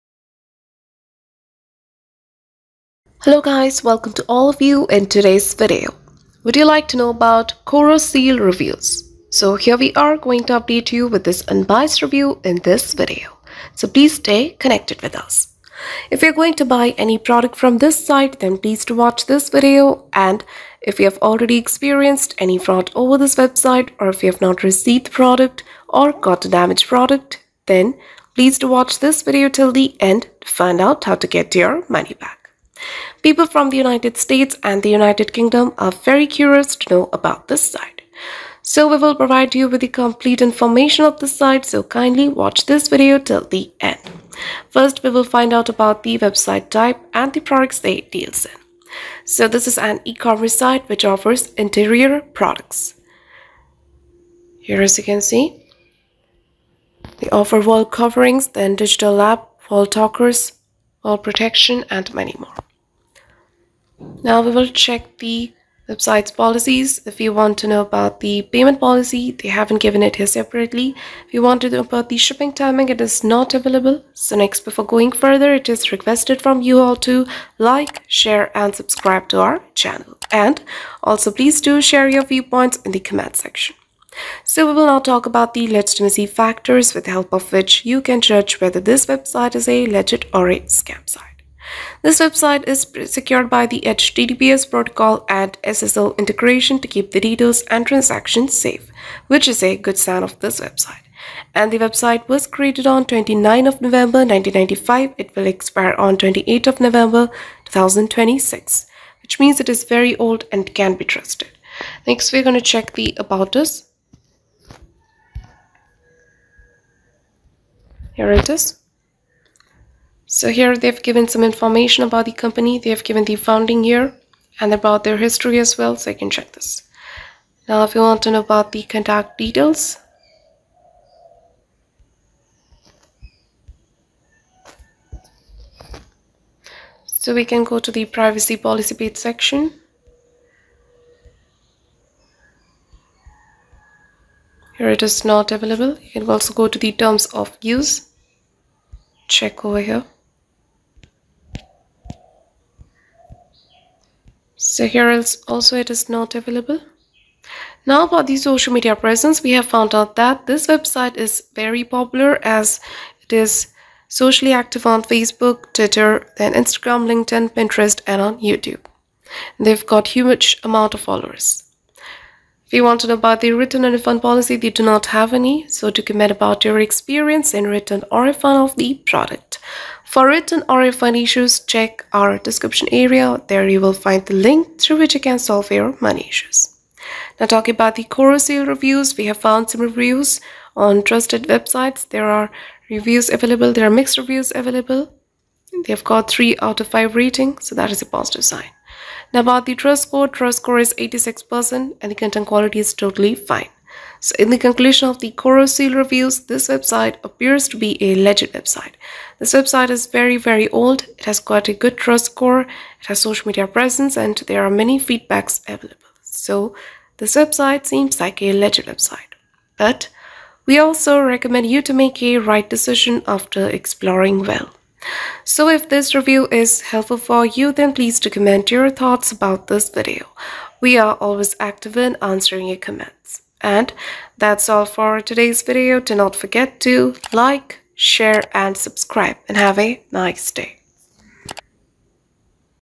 Koroseal Reviews (Aug 2023) - Is This A Fake Website? Find Out! | Scam Inspecter
Do you want to know about Koroseal Reviews?  Watch this video till end because in this video you will get to know about the legitimacy of the site.

Address: 3875 Embassy Parkway, Suite 110 Fairlawn, Ohio 44333

Scam Inspecter